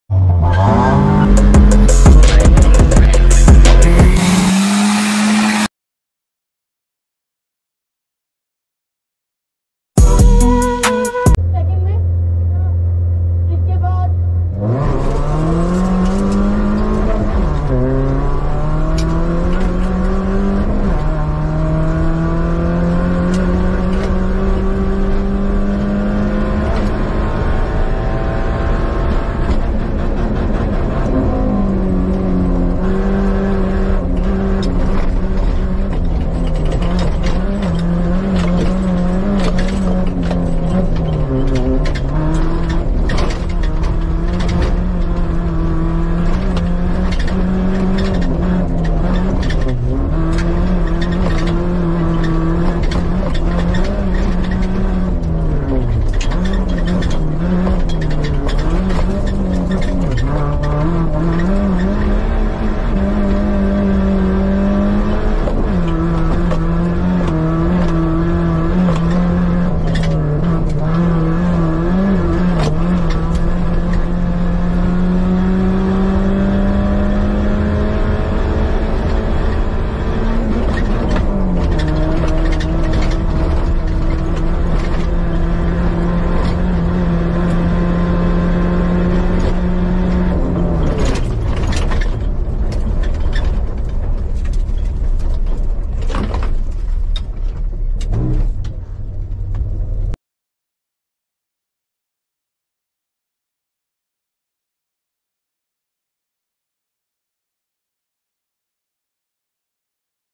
Full video Cholistan rally Qualifying onboard..
Gopro view of qualifying Derajat jeep rally where umar kanju secure his podium ...